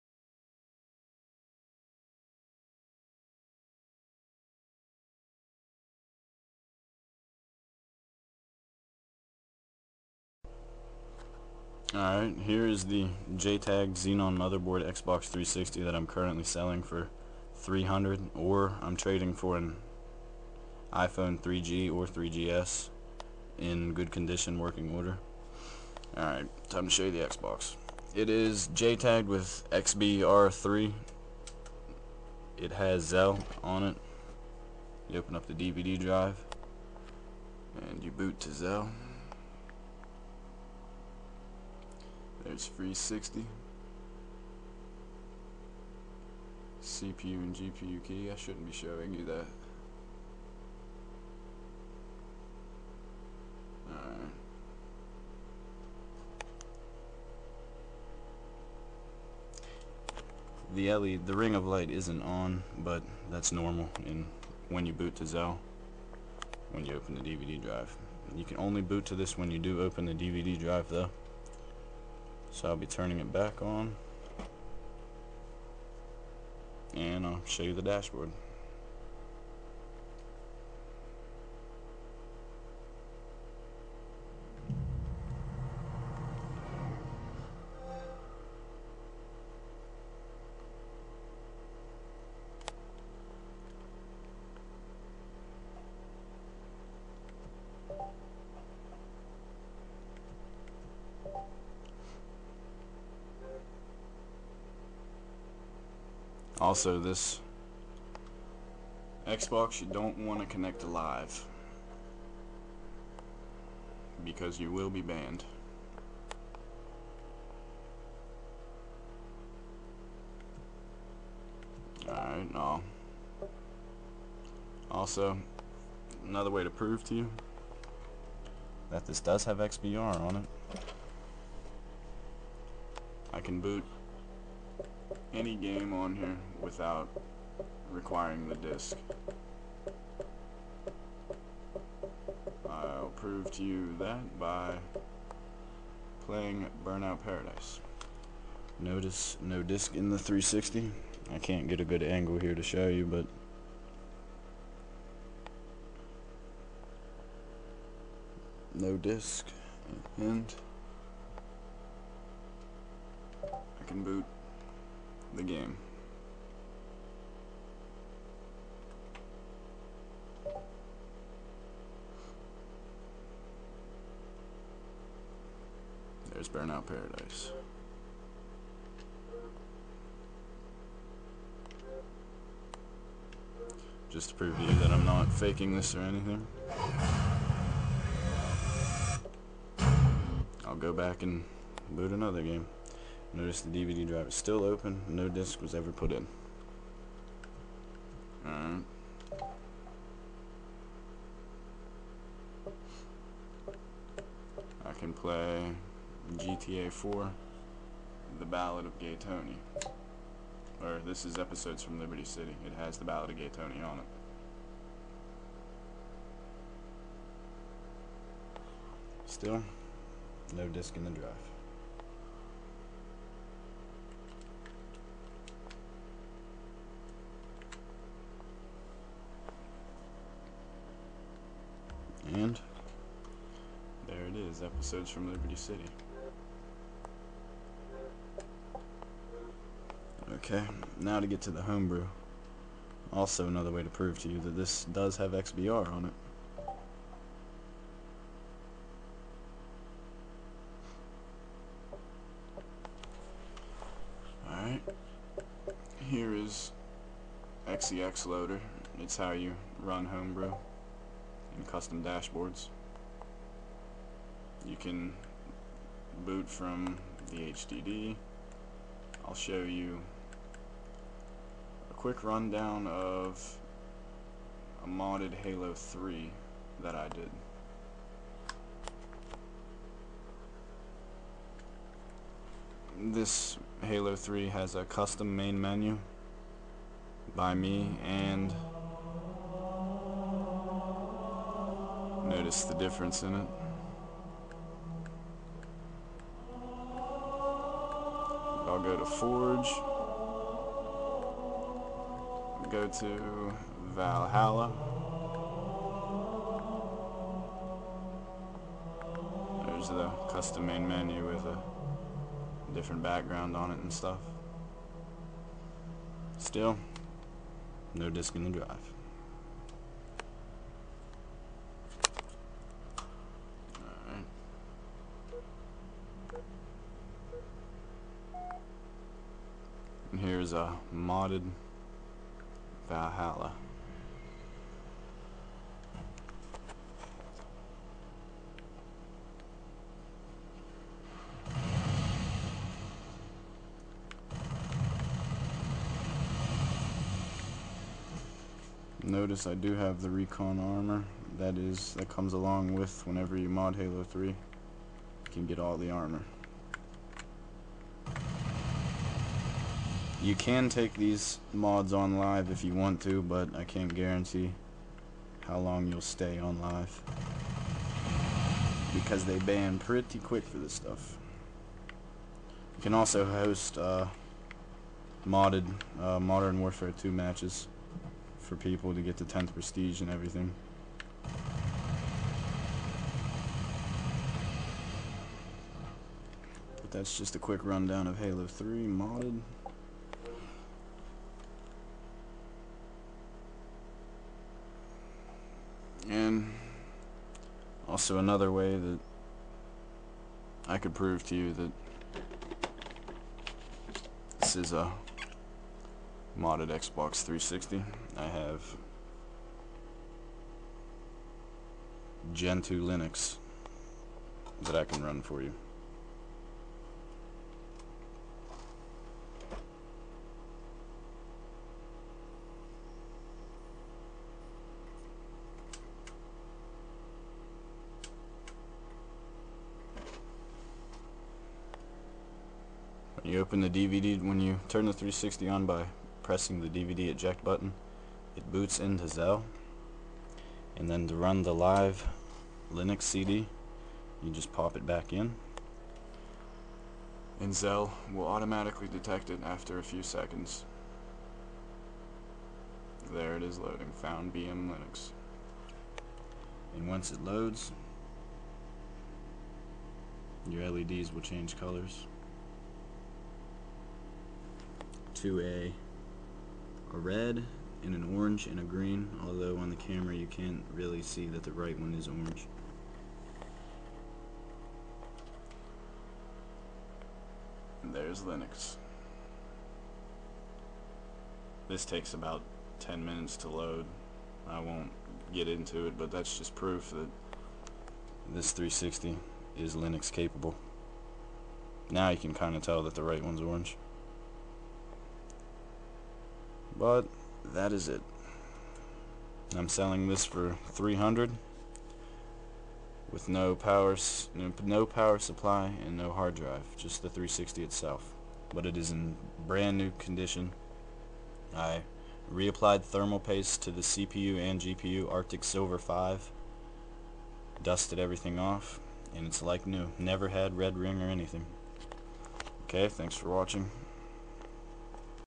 Xenon 4 Xbox 360 with Xbr3.
This is a reflow of the XBox 360 GPU with real time process monitoring and data capture. This information is captred to creat a more accurat reflow profil...